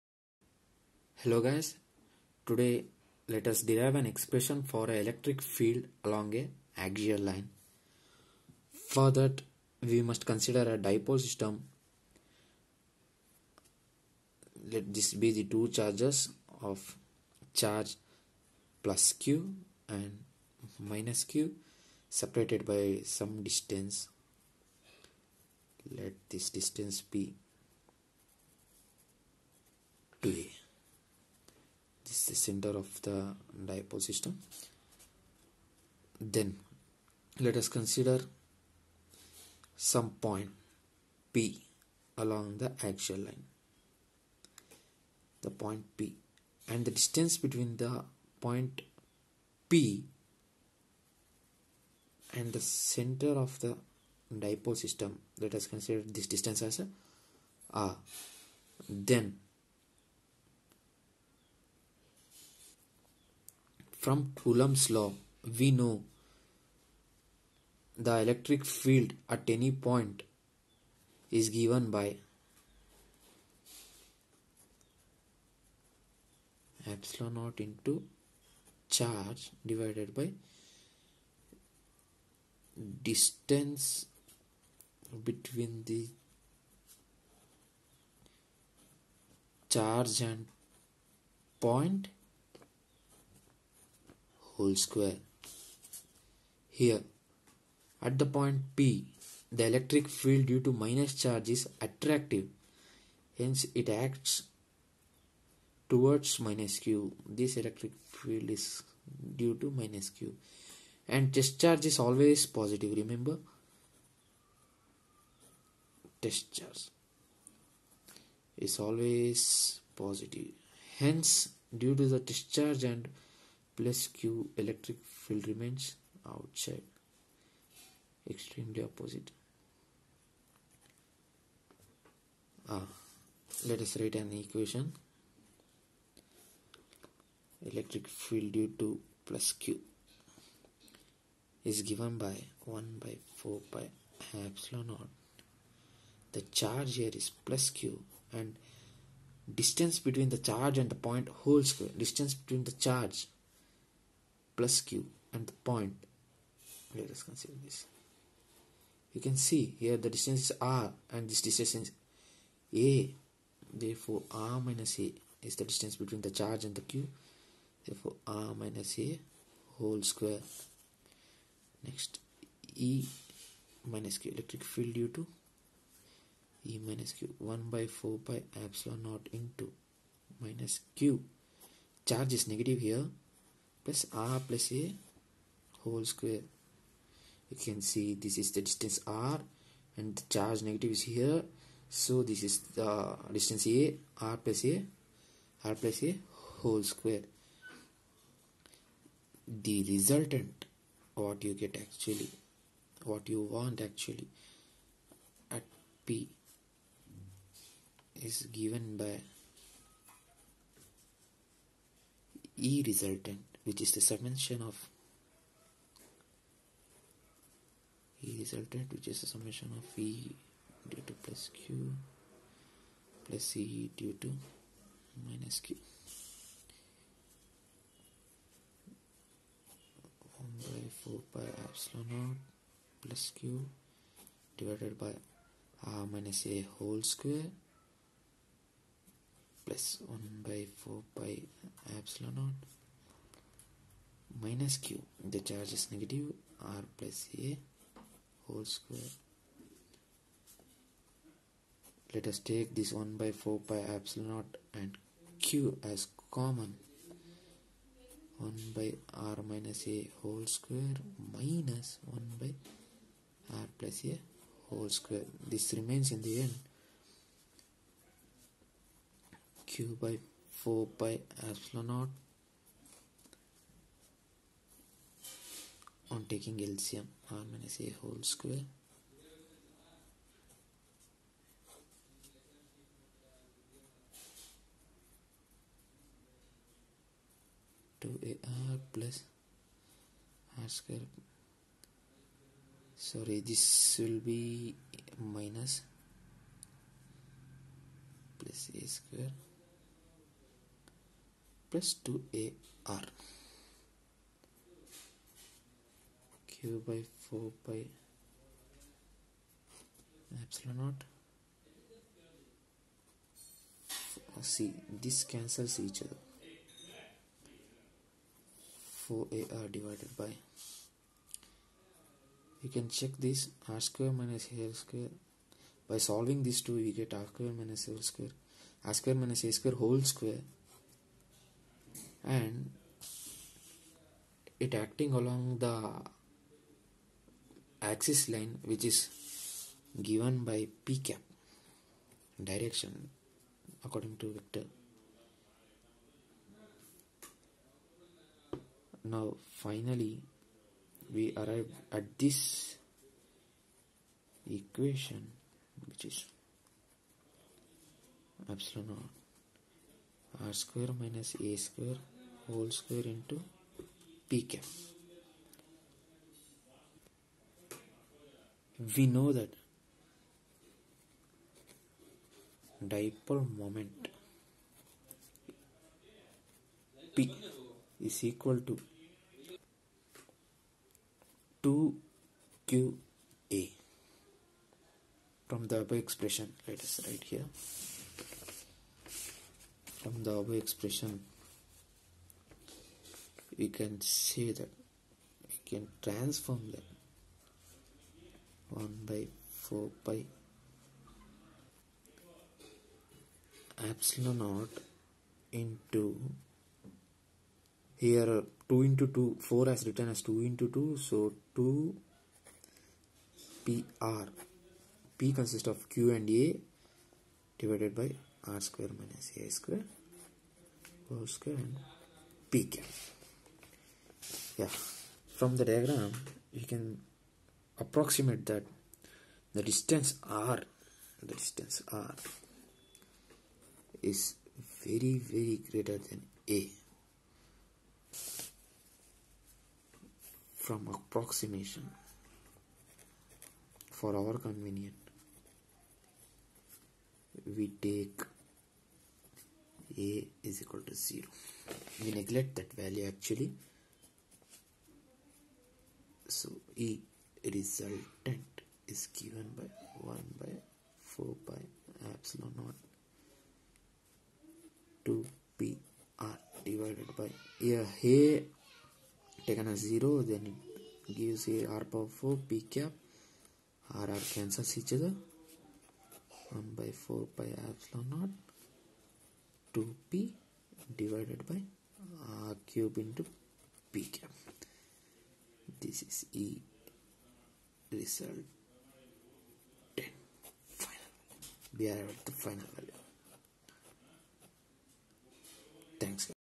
Ncert 2nd Pu 1st chapter physics derivation of electric field at a point on axial line.
Physics 2nd pu electric field derivation

Comment down your views, Doubts, Any suggestion or opinion!!